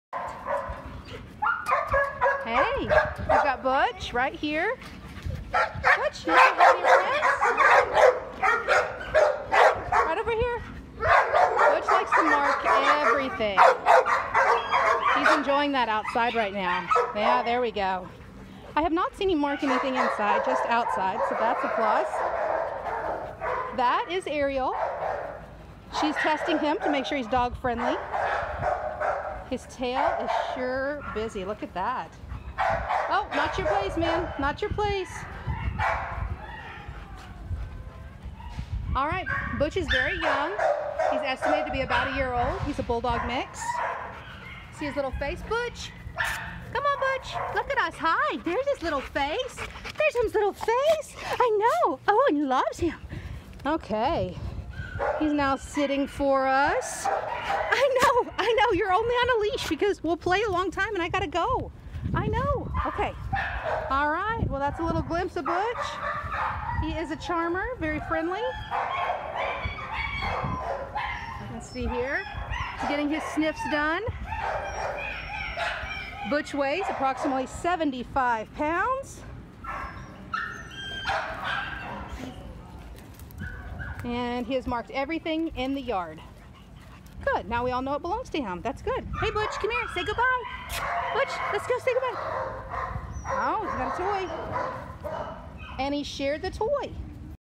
Butch - Sand Springs Animal Welfare
Butch, one-year-old, bulldog, male, dog friendly, very people friendly, sits on command and loves attention - Sand Springs Oklahoma
* Shown in the video with Ariel